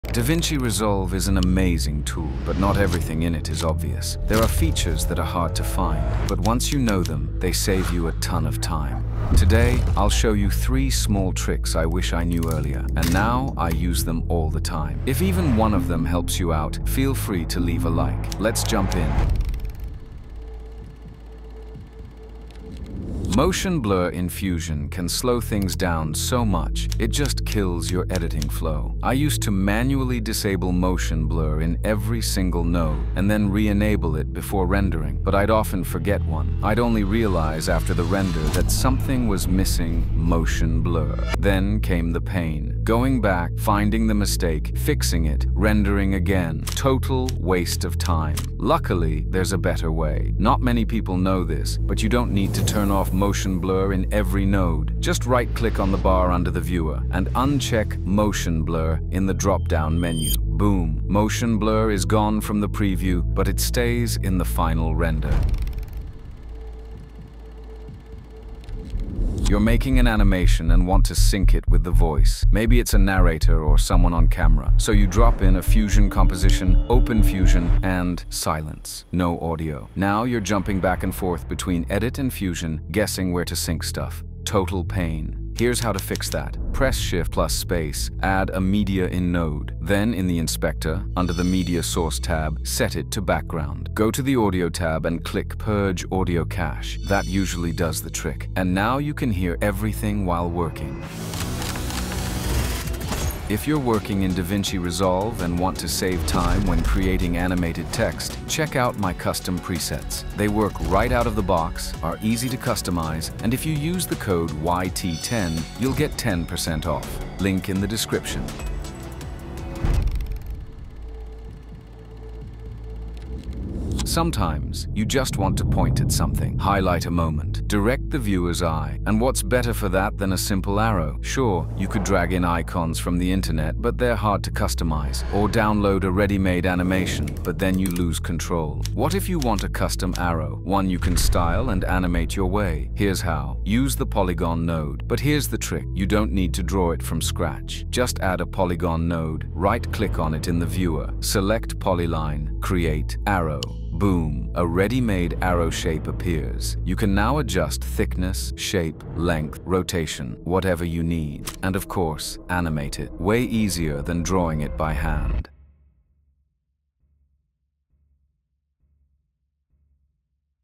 3 Resolve Hacks That Instantly Speed Up Your Workflow
⭐️ Grab my 46 ready-to-use text animations for DaVinci Resolve and speed up your workflow:

🚀 In This Video:
DaVinci Resolve is full of hidden features — but some of the best ones are buried deep.
Today, I’ll show you 3 small tricks that completely changed my workflow:
✔️ Disable motion blur in previews (but keep it in the final render)
✔️ Get audio into Fusion directly from your timeline
✔️ Create customizable arrows instantly with the Polygon node

These aren’t flashy effects — they’re real time-saving tricks that make editing and animation smoother, faster, and way less frustrating. Perfect for editors and motion designers working in Resolve and Fusion.

🎬 Timestamps:
00:00 Intro
00:20 Disable Motion Blur in Preview
01:05 Get Audio Into Fusion
02:00 Quick Arrows Using Polygon Shapes